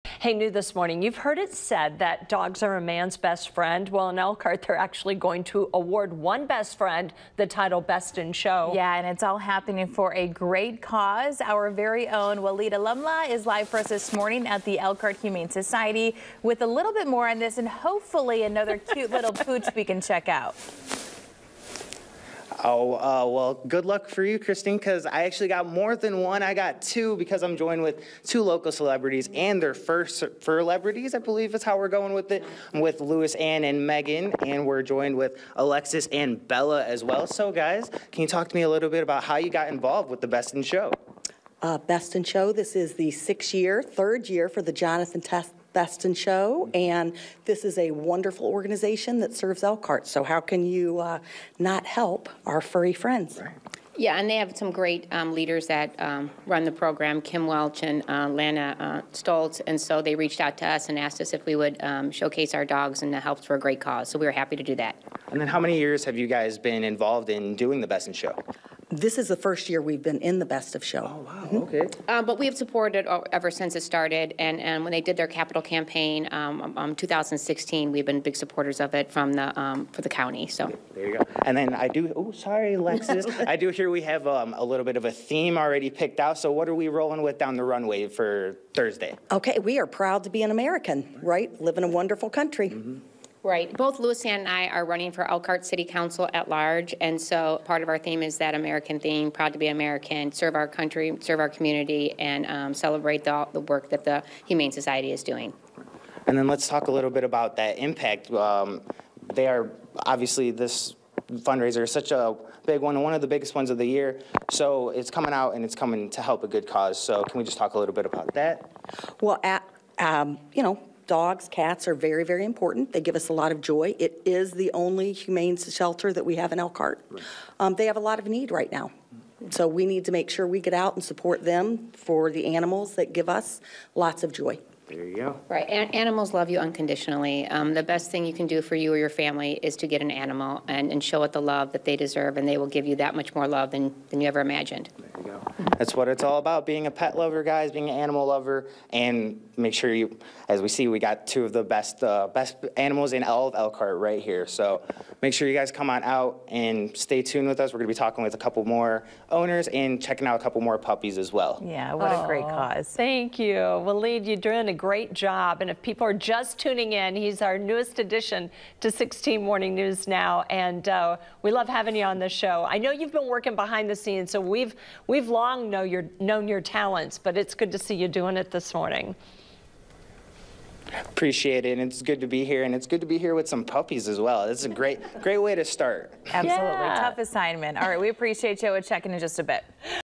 Humane Society of Elkhart County prepares for annual ‘Best in Show’ fundraiser - Pt. 1
The HSEC is hosting its sixth annual “Best in Show,” which is the shelter’s biggest fundraiser of the year, on Thursday!